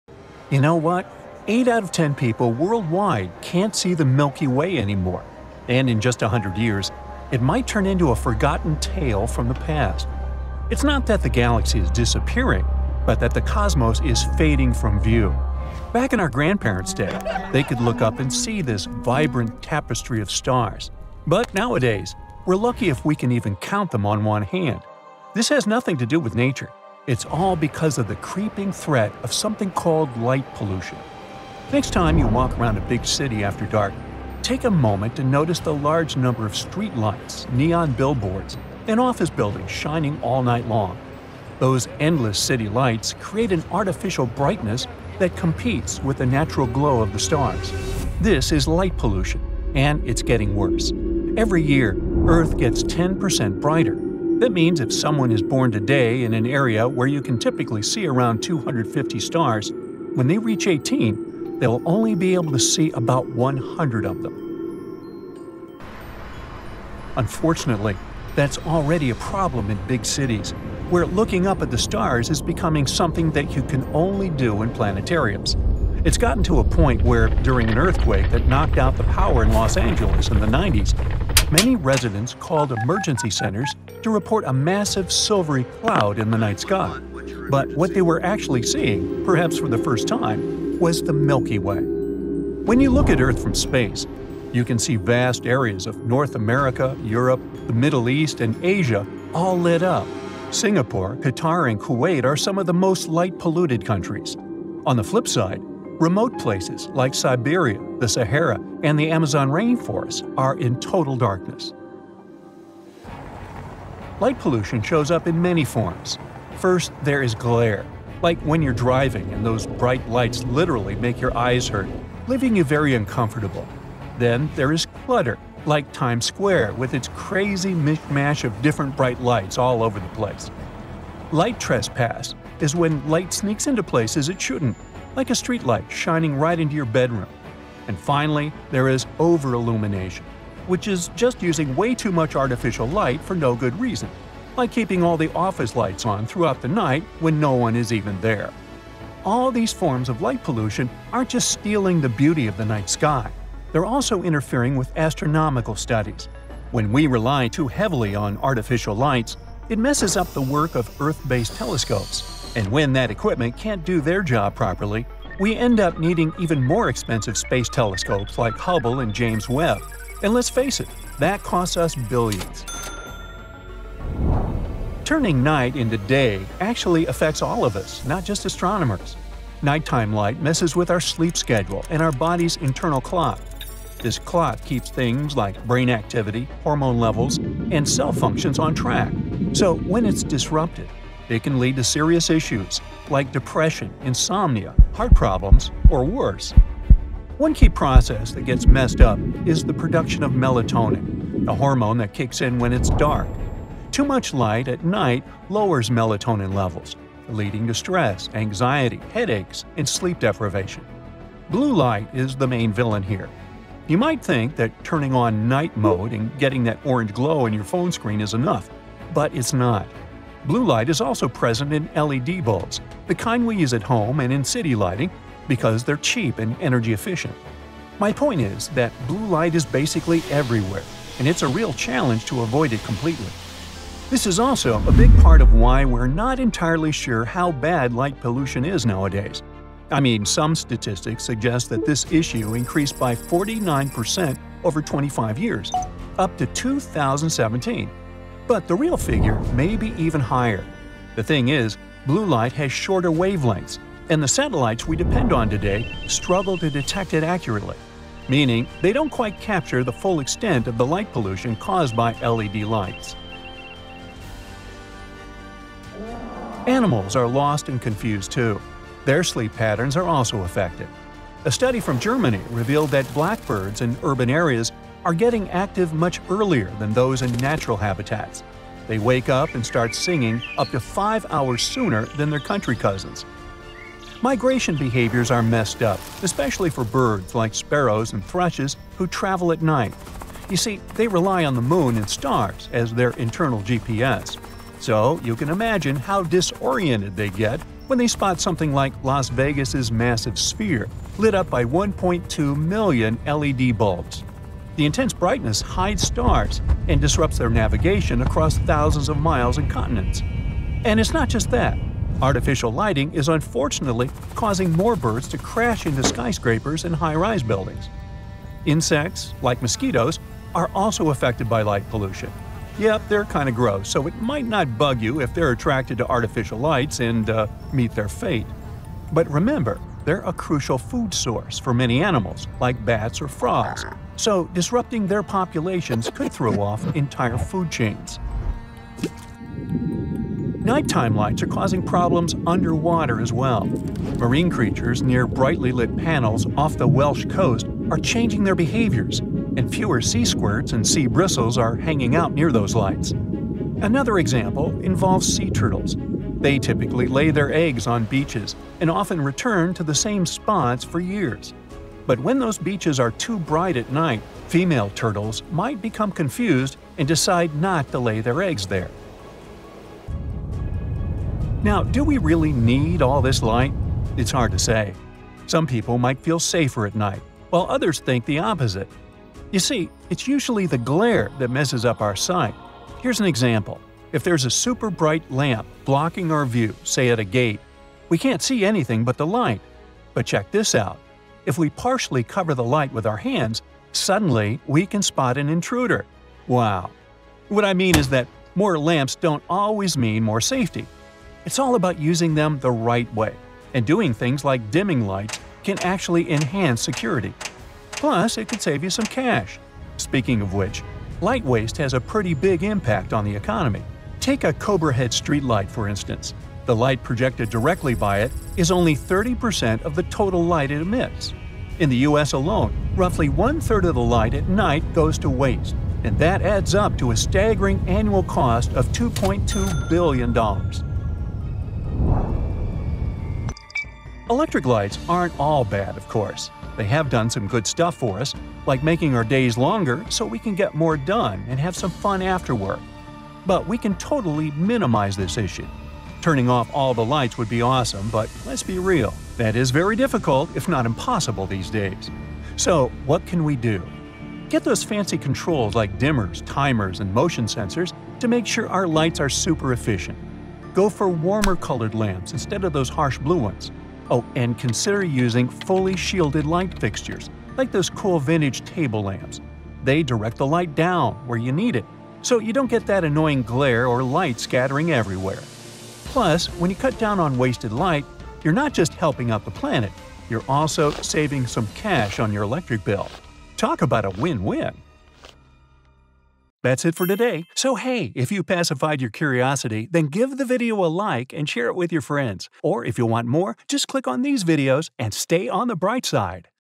NASA Says We Are Losing Stars in Our Skies
Did you know that the beautiful Milky Way might soon disappear from our night sky? Light pollution, caused by all the bright lights in cities, is making the stars harder to see. Scientists say that in just 20 years, most people on Earth won’t be able to spot the Milky Way without special trips to remote areas. It’s like losing a piece of cosmic history right above our heads! Luckily, some places are working to reduce light pollution by using dimmer, shielded lights to protect the view. If we act now, future generations might still get to enjoy stargazing and the magic of our galaxy. 🌌✨